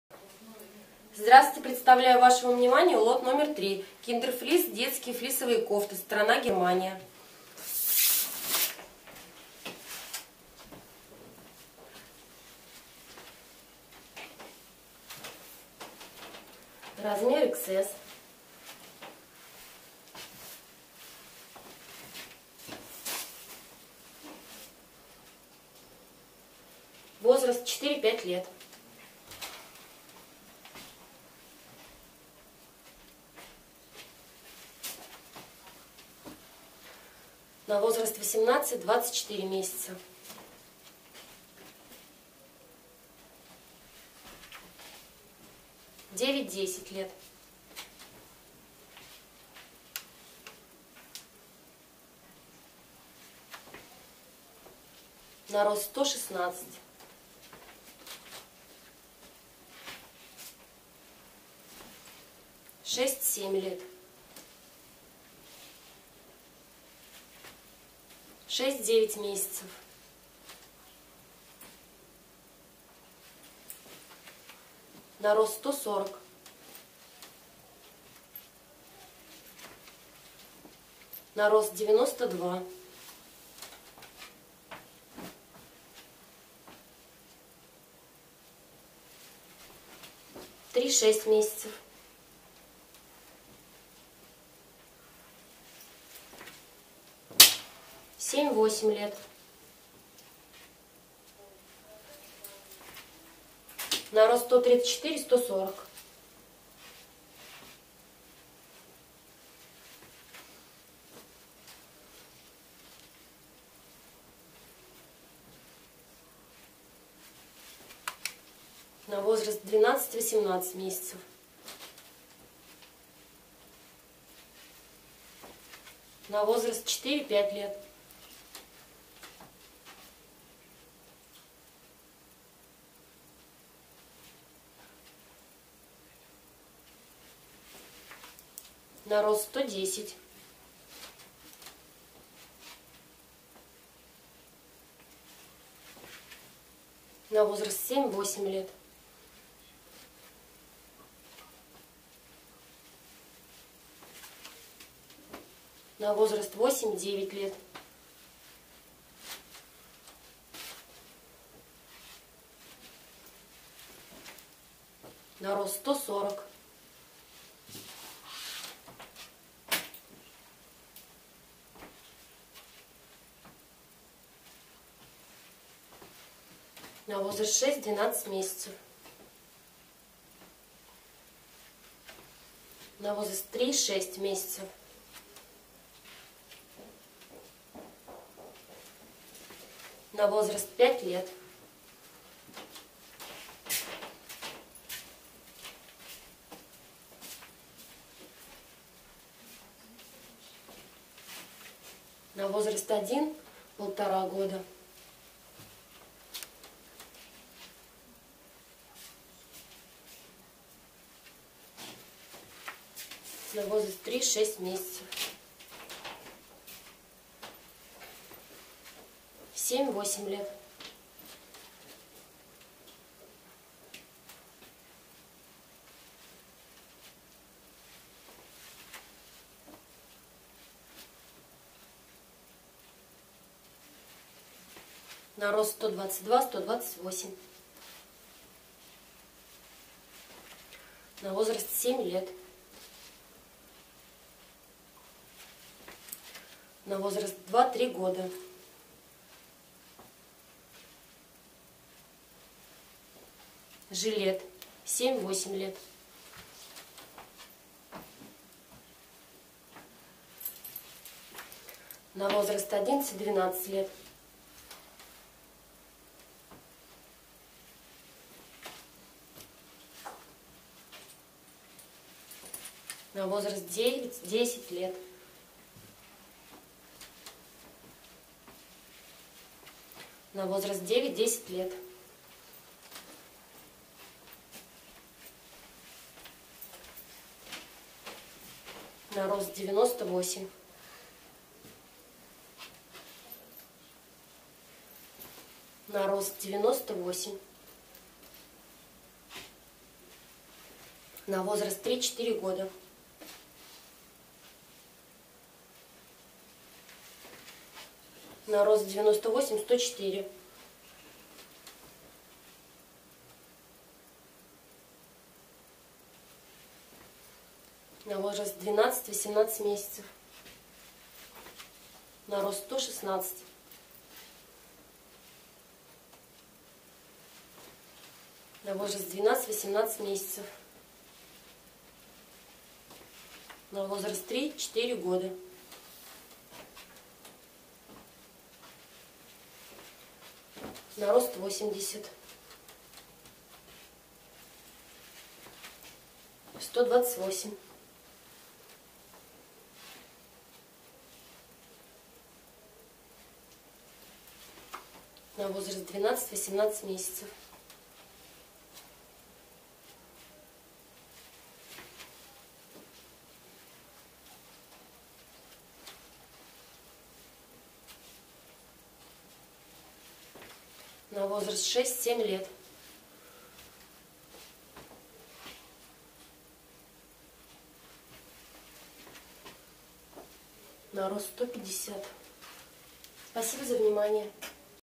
KINDER FLEESE 3, секонд хенд
KINDER fleese (лот № 3)
Страна: Англия
Сезон: Демисезон
Детские флисовые кофты (секонд хенд)
Цена за кг.:7.56 евро
Цена за мешок: 75.6 евро
Цена за ед.: 1.68 евро